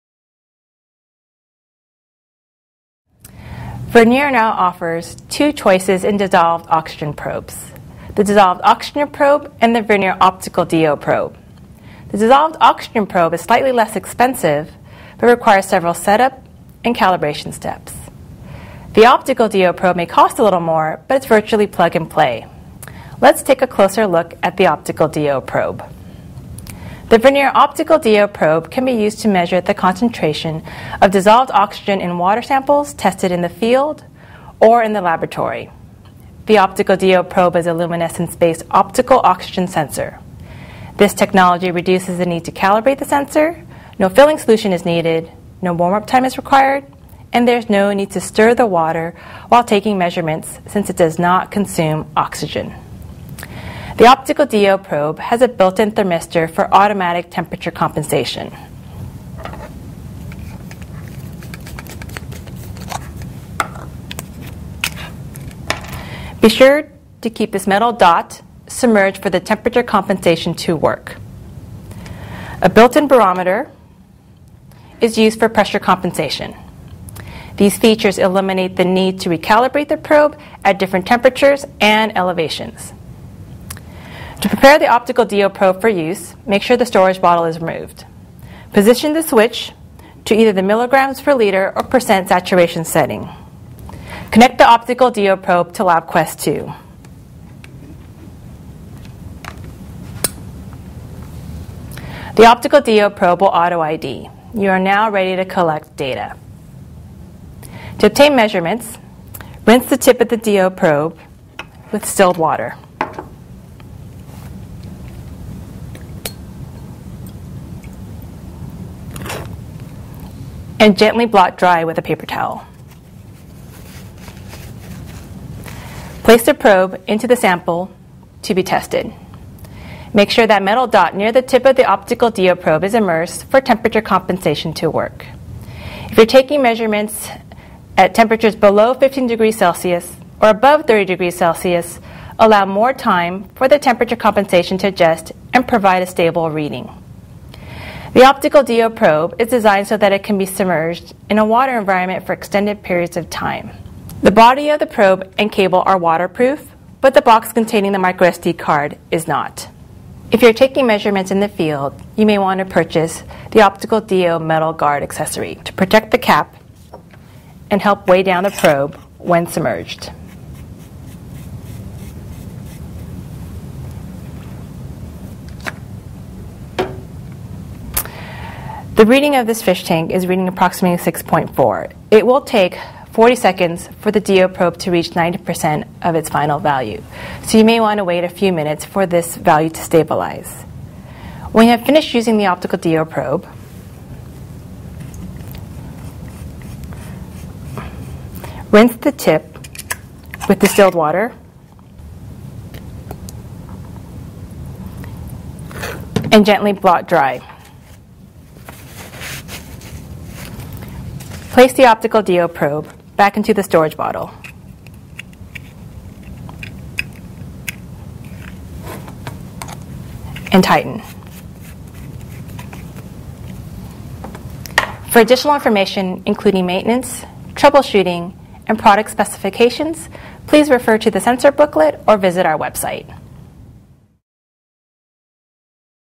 016 Vernier ODO最新光學溶氧感應器示範影片 廣天國際有限公司翻譯
Vernier2013年春季最新上市的光學溶氧感應器,其功能已超越了傳統的溶氧感應器,晶片設計也超過工業用的水準.
不僅不需要暖機,校正,還有自動溫度和壓力補償.這對過去使用溶氧感應器痛苦連天的老師們.
實在是一大技術上的突破呢!!

更多訊息盡在
Vernier Aisa FB